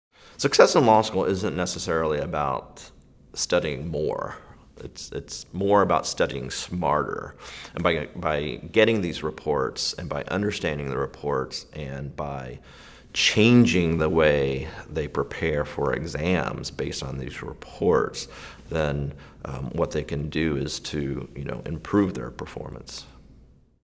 Using ExamSoft Reports to Improve Study Habits
Tommy Sangchompuphen, Associate Dean of Student Learning and Assessment and Associate Professor of Law atLincoln Memorial University Duncan School of Law, shares how ExamSoft reports allow students to understand their weak areas and adjust study habits accordingly.